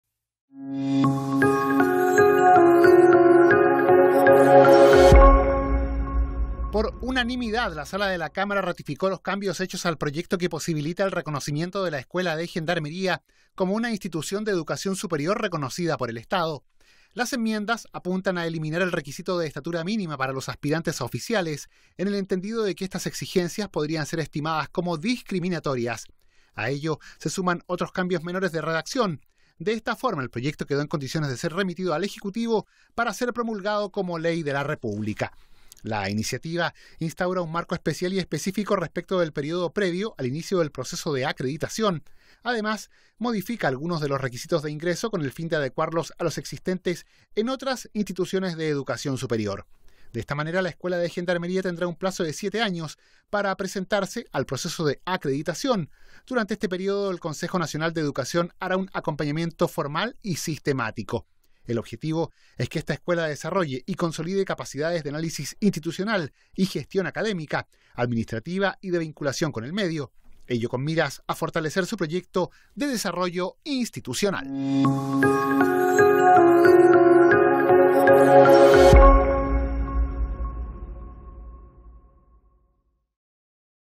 Escuela de Gendarmería operará como institución de educación superior
La iniciativa, despachada a ley, fortalece la instrucción de los gendarmes, mejora las mallas curriculares y profesionaliza la actividad de las y los funcionarios.